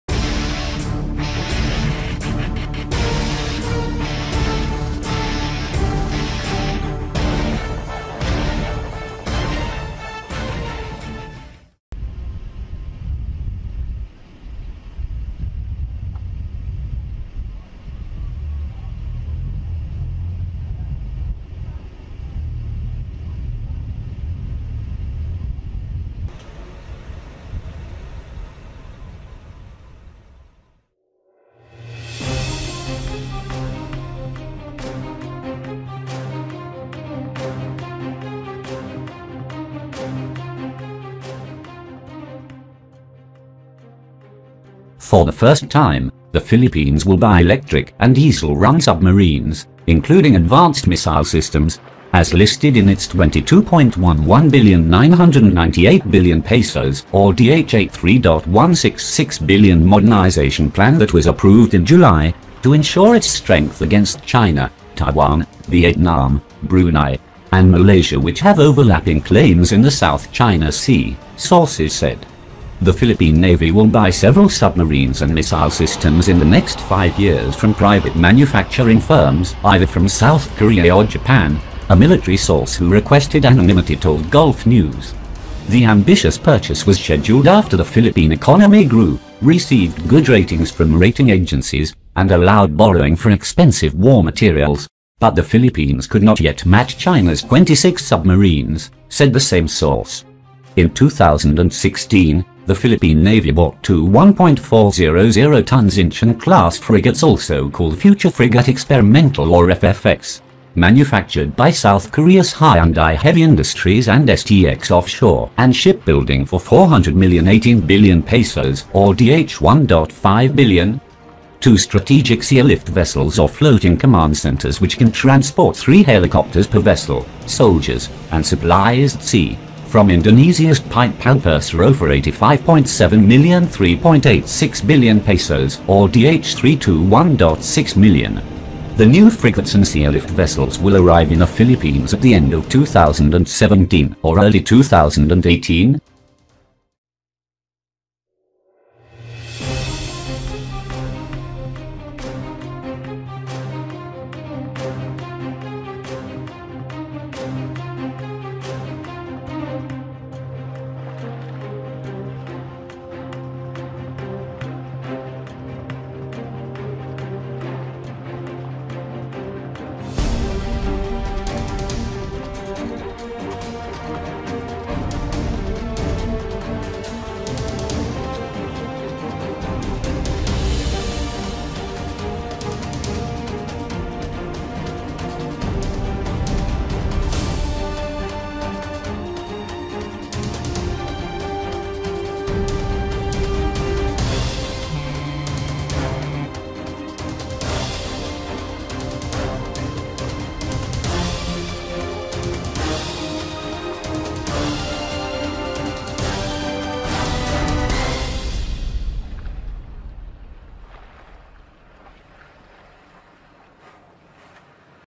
필리핀, SKR / 일본의 디젤 엔진 잠수함 구매 확인
The Philippine Navy will buy several submarines and missile systems in the next five years from private manufacturing firms either from South Korea or Japan,” a military source who requested anonymity told Gulf News.

“The ambitious purchase was scheduled after the Philippine economy grew, received good ratings from rating agencies, and allowed borrowing for expensive war-materials, but the Philippines could not yet match China’s 26 submarines,” said the same source.